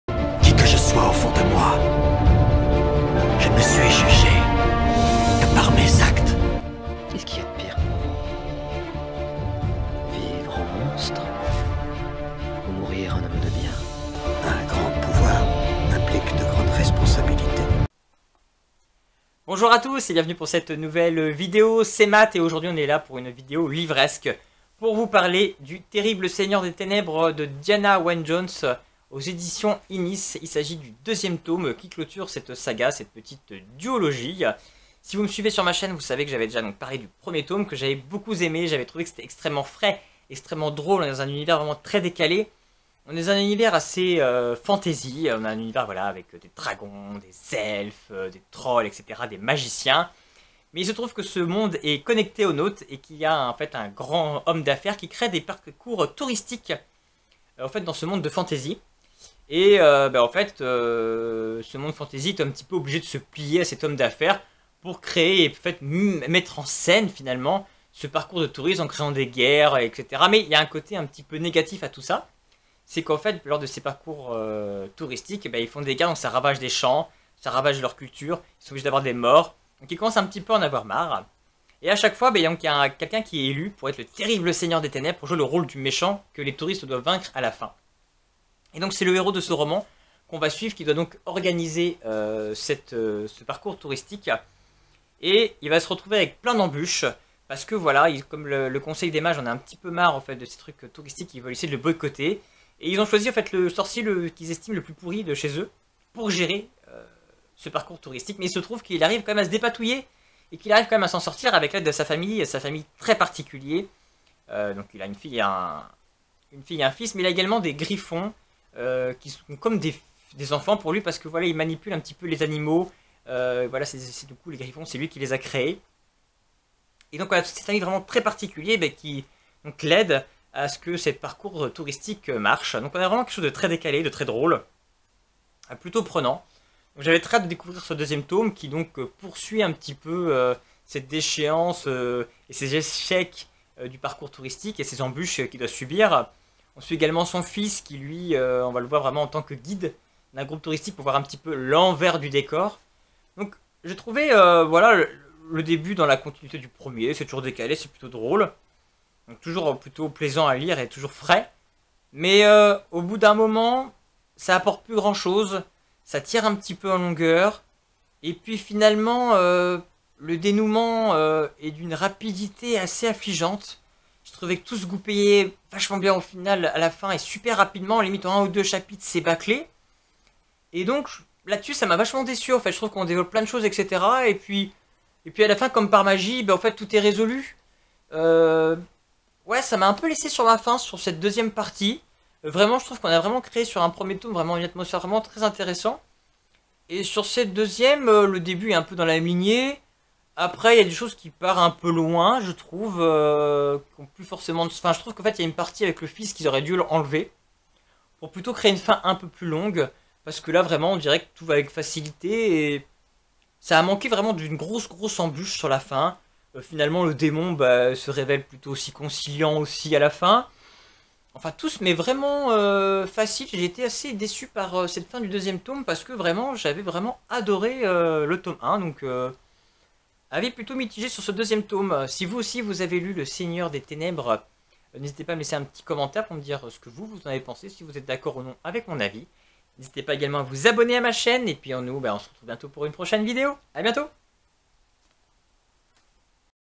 le terrible seigneur des ténèbres tome 2
bonjour

voici mon avis sur le tome 2 du terrible seigneur des ténèbres ches Ynnis editions